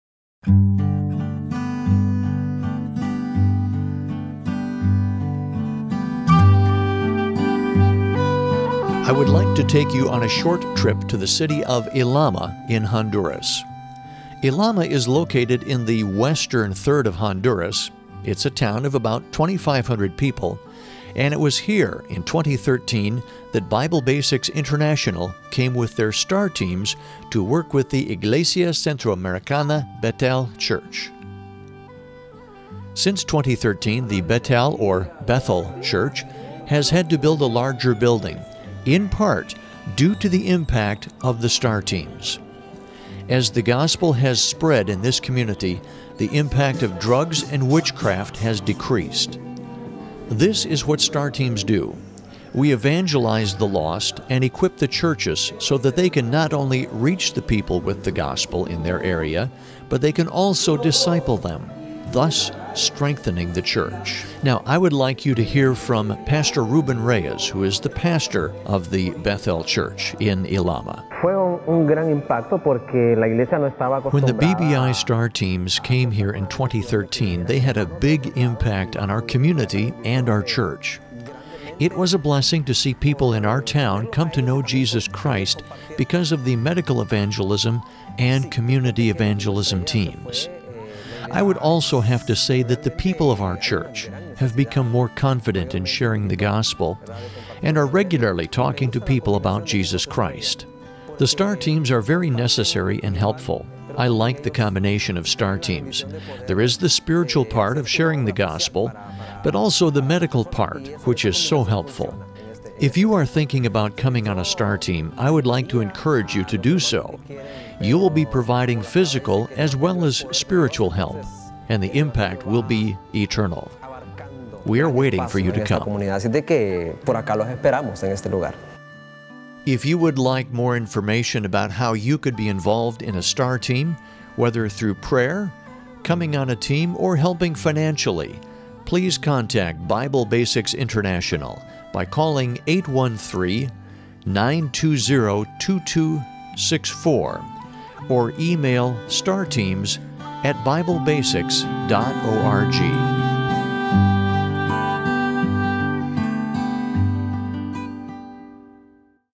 Short Term Missions Impact
I would like to show you the impact the BBI STAR Teams have in a community. We will visit the city of Ilama, Honduras to understand how the STAR Teams have benefited that city.